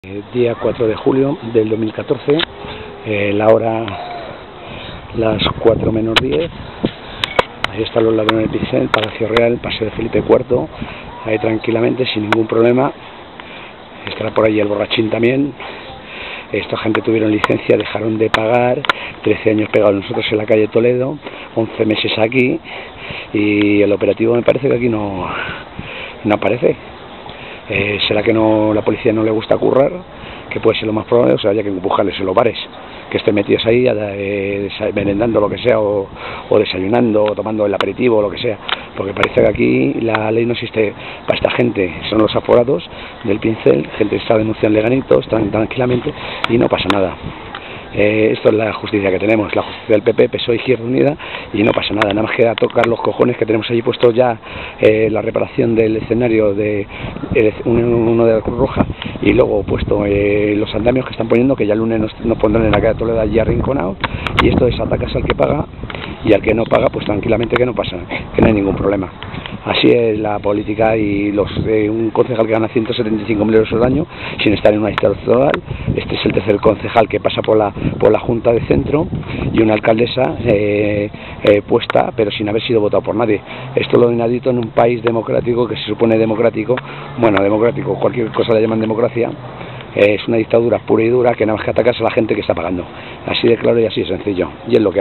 viernes 4 julio 2014.15.50 h de la tarde,los ladrones del pincel palacio real, madrid
viernes4 julio 2014,15,50 h de la tarde los ladrones del pincel y grafito palacio real de madrid españa,denunciados en leganitos por amenazas de muerte y extorsiones,cobran para ,pemsiones .remi,etc,hacen fraude fiscal hacienda ,estado,ayuntamiento.tuvieron licencia en plaza mayor,por tomarplaza,lista y situado a dedo,13 años calle toledo,11 meses aqui y el seguimiento no aparece por ningun lado de la policia municipal